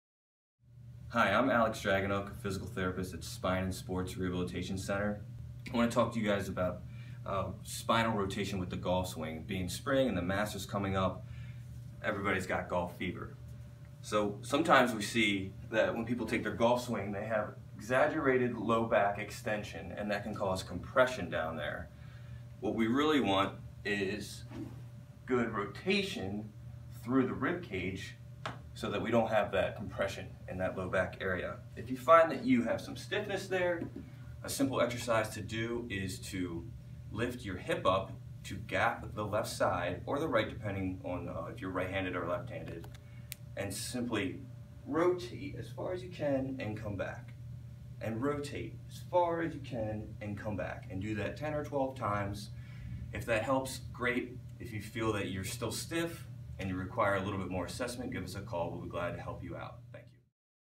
Golf Tips for a Healthier Spine
Dr. Alex Dragonuk demonstrates a few tips to reduced stress on the spine in the golf swing.